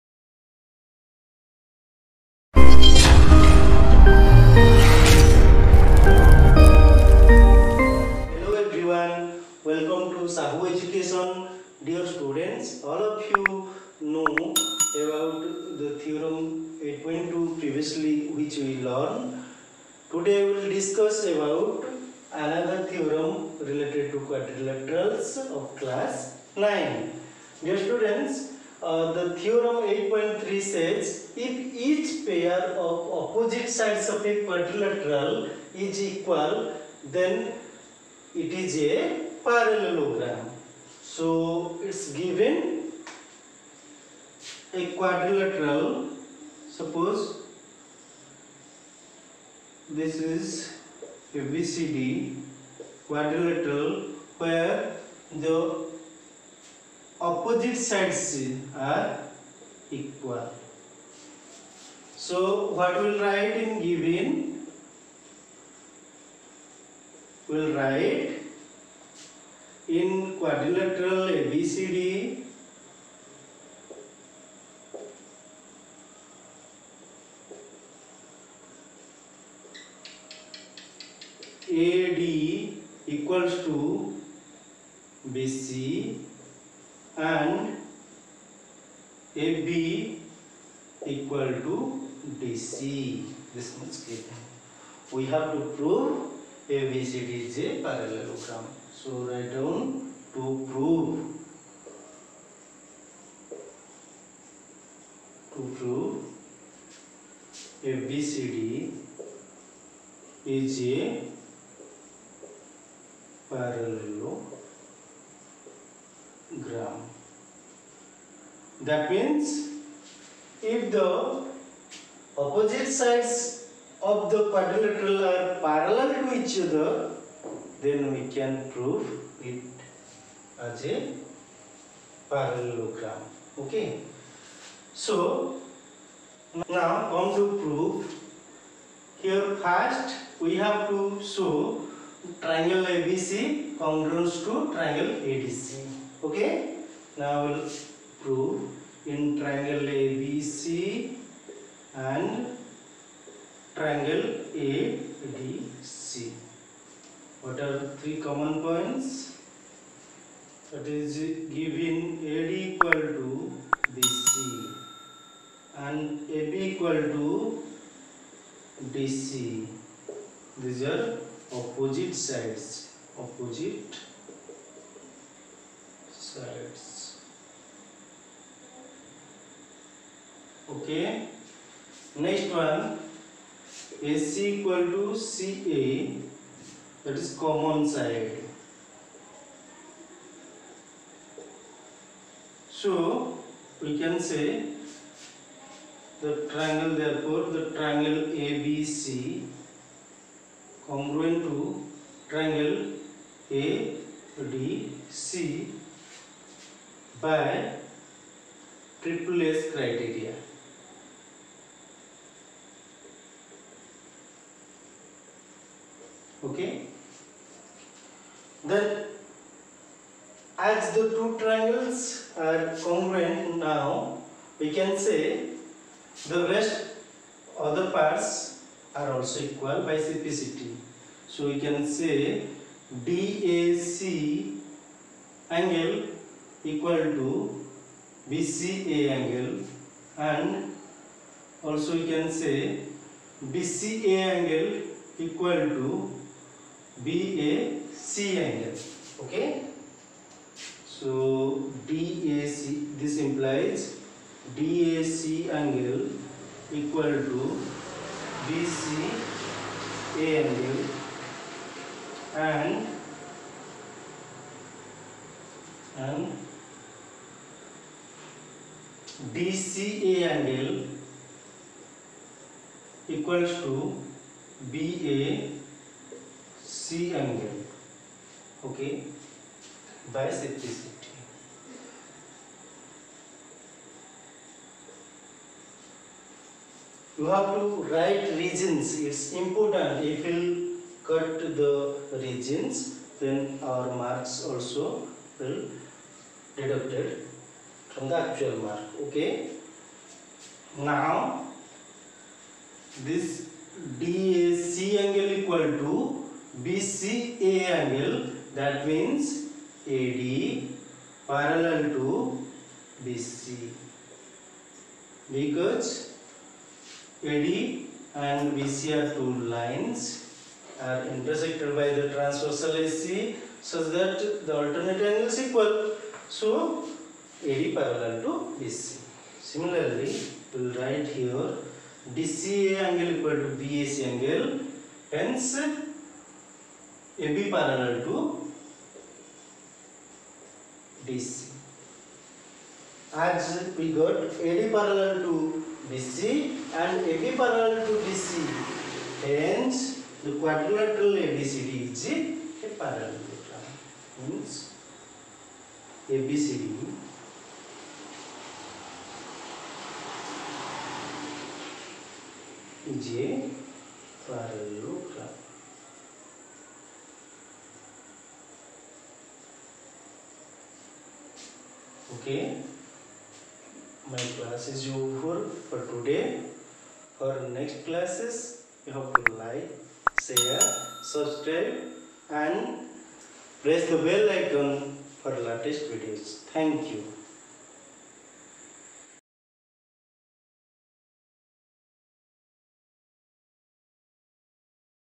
Quadrilaterals#theorm-8.3#class-9#std-9#part-4#sahoo education
Hi everyone
In this video I discussed about the theorm-8.3 briefly.
So check it for proof.